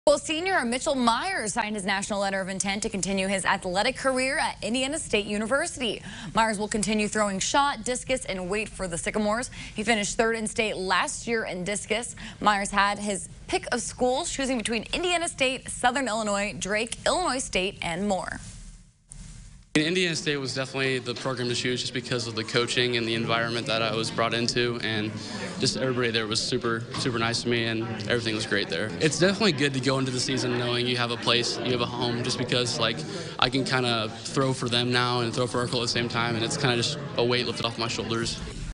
Arcola’s Mitchel Myers signs with ISU
Arcola senior Mitchel Myers signed his nation letter of intent to continue his athletic career at Indiana State University.